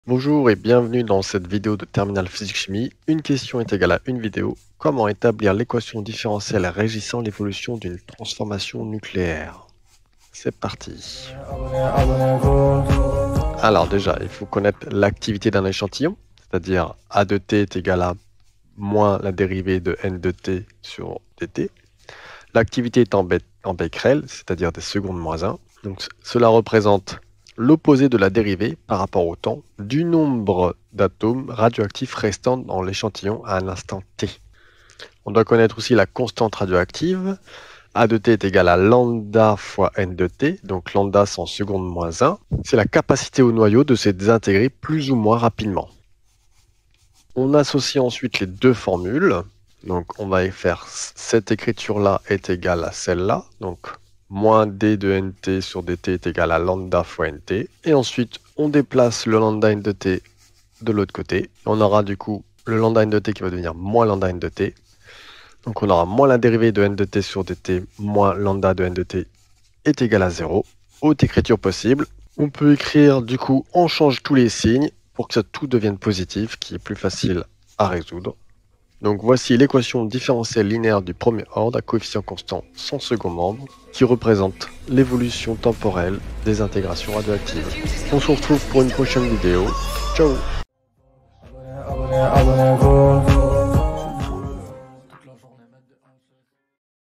Établir l’équation différentielle régissant l’évolution d’une transformation nucléaire(1Q=1V) 1q=1v
1 question = 1 vidéo sur  " Établir l’équation différentielle régissant l’évolution d’une transformation nucléaire"
Question BAC 2024 physique-chimie
- Activité